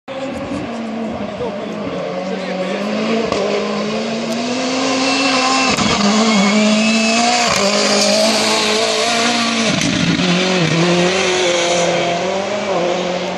Moris Cup Jahodna 2013 Martin Koiš Ford Fiesta
Action-Moris Cup Jahodna 2013
Rider-Martin Koiš
Car-Ford Fiesta